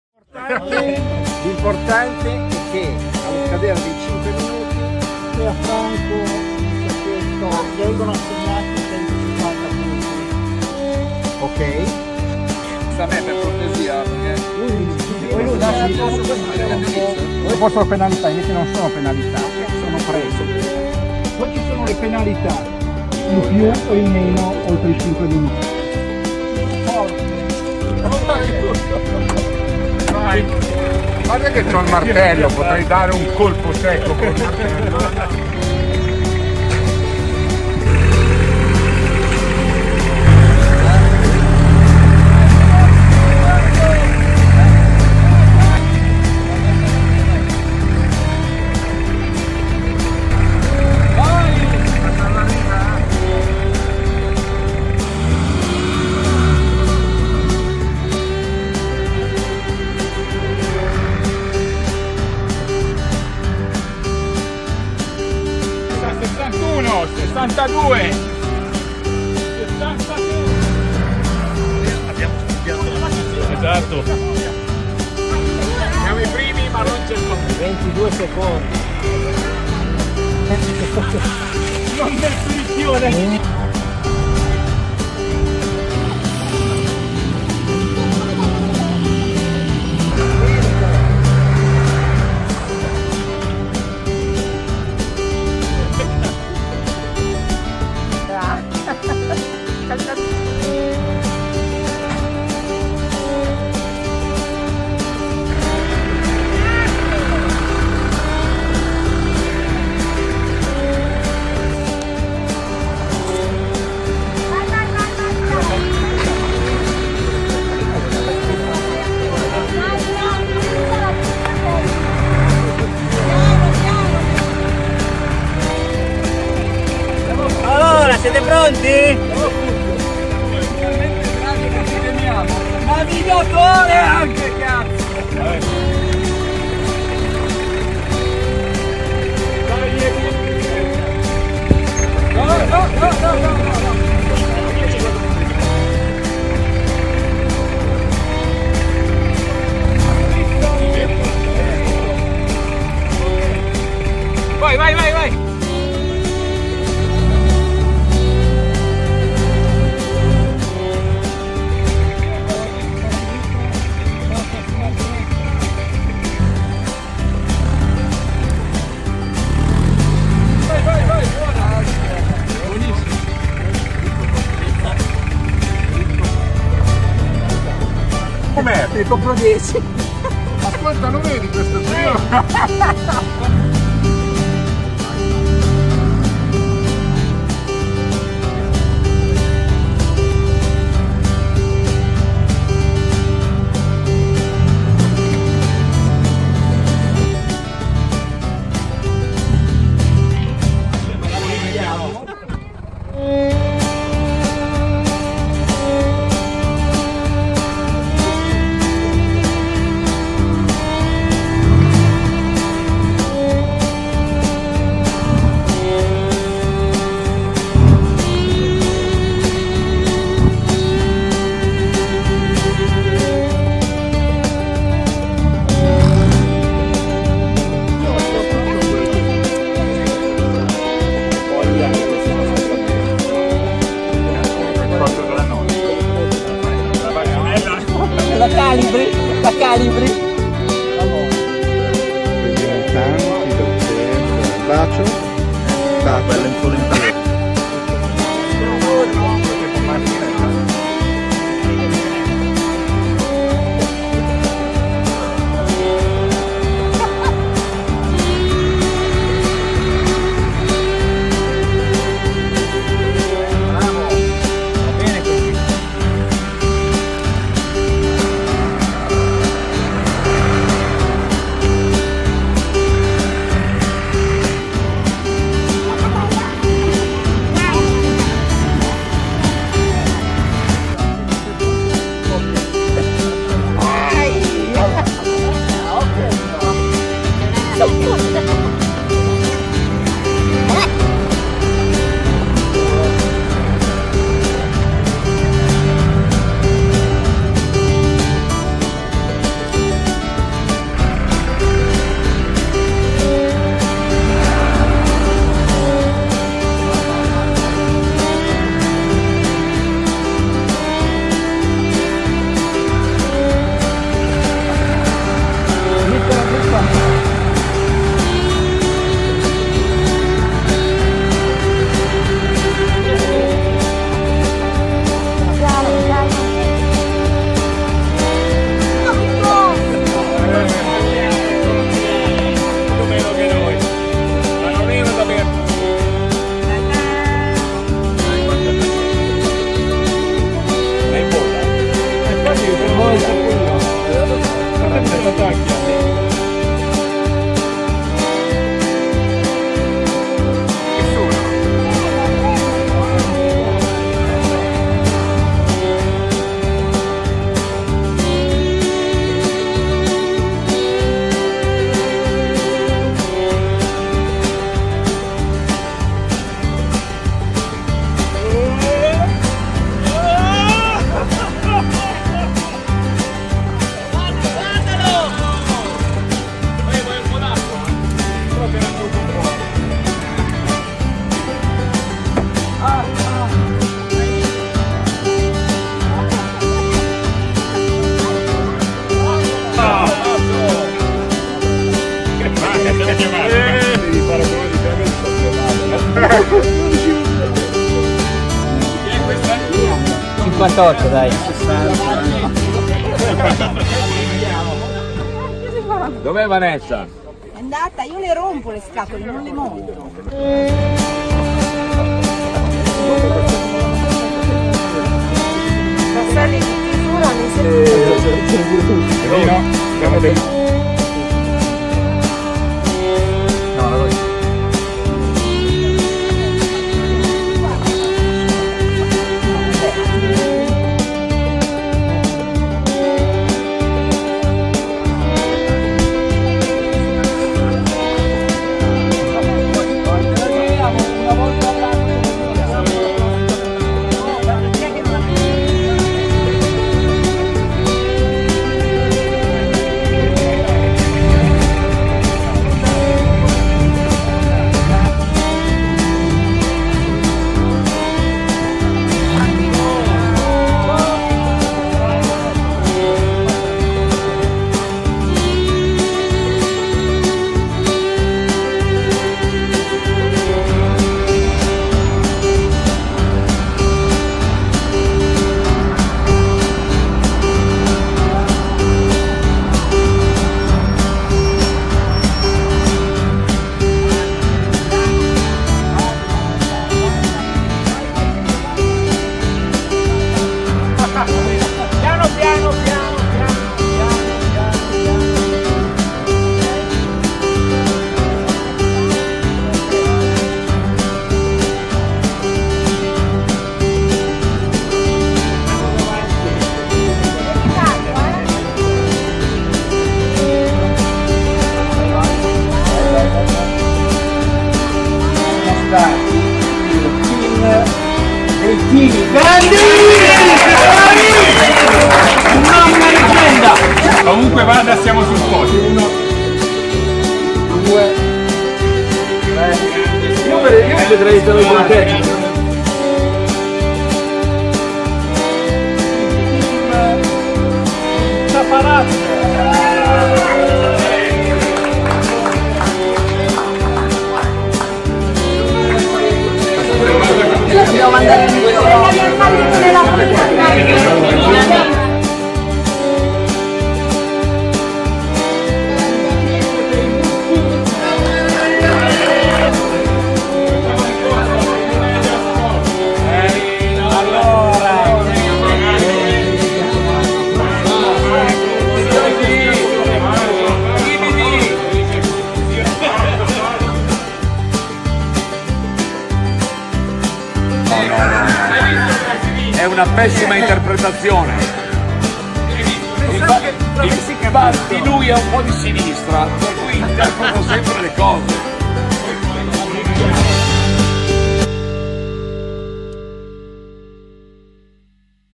Summer Team Building 2019 3
Evento Sociale Summer Team Building - settembre 2019, pista di Corbetta